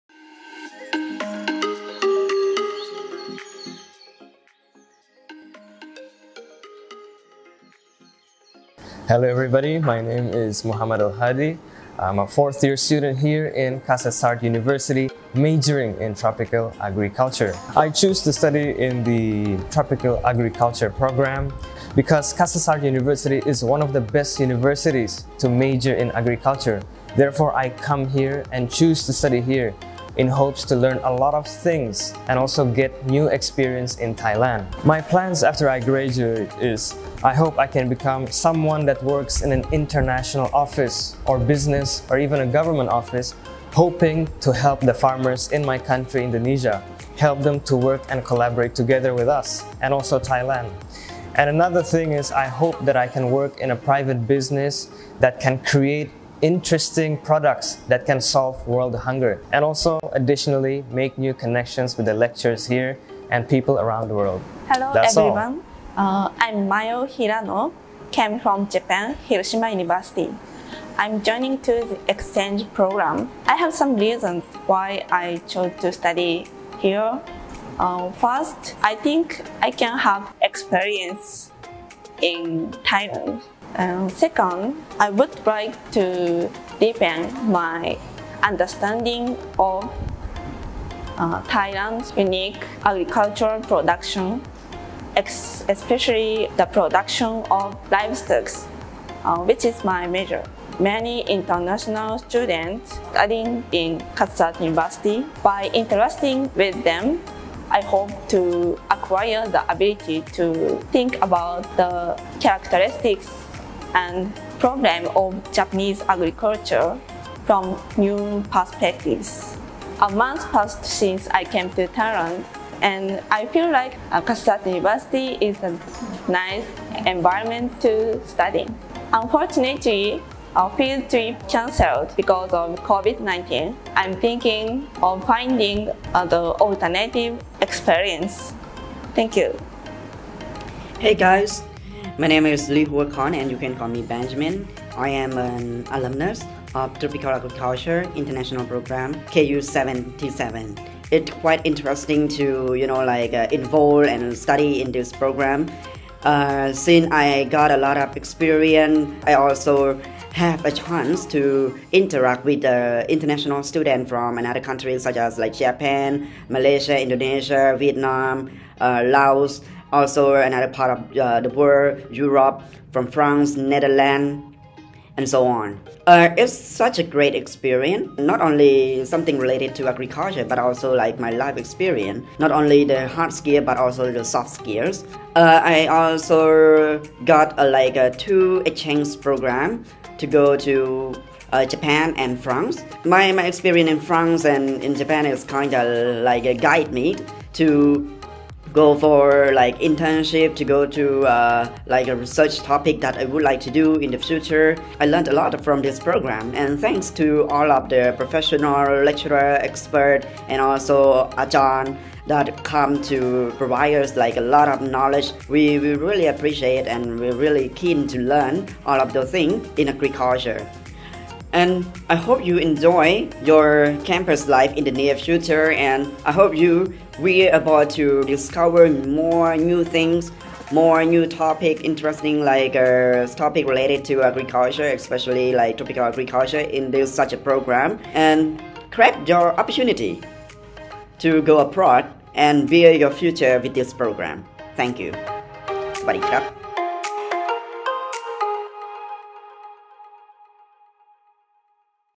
Why Tropical Agriculture International Program Ep.1
Comments from our students...Why Tropical Agriculture (International Program) @ Faculty of Agriculture, Kasetsart University